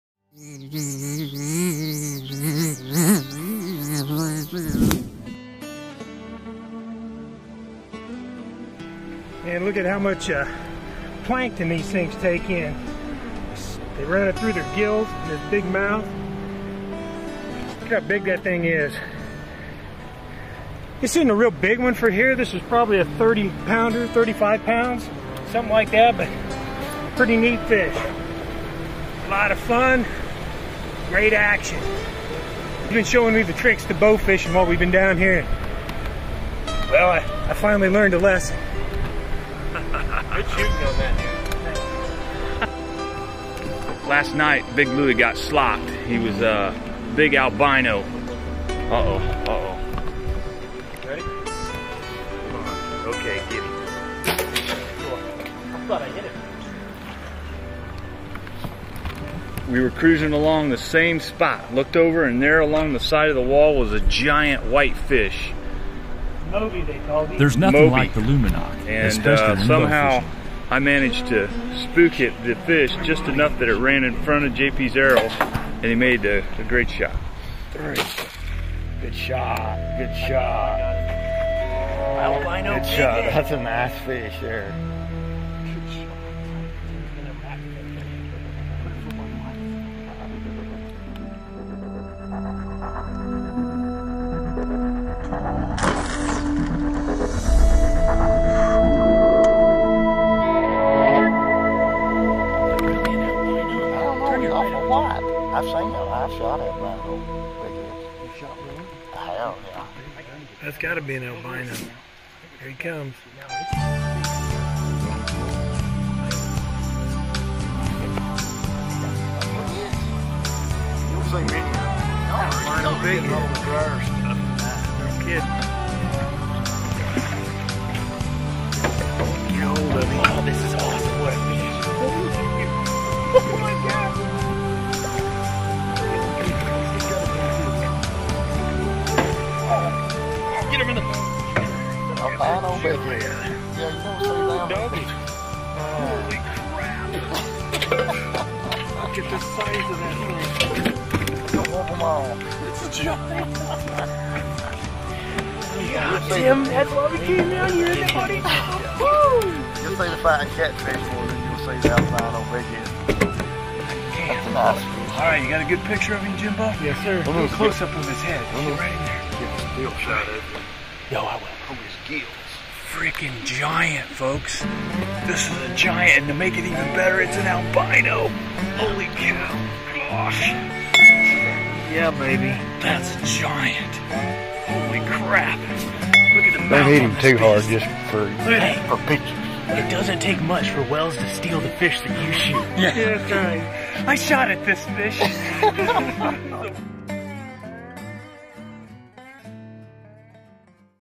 RIESEN-WEIß MOBY STEIGT DANN STIRBT ALS ZWEI BRENNENDEN PFEILE SIND GESTARTET
Ein riesiger weißer großer Kopf steigt aus der Tiefe nur mit kollidieren Feuer angezündet Pfeile. Finden Sie diese seltene albino-Trophäe genommen wird von JP Morris. Wie Tim Wells face book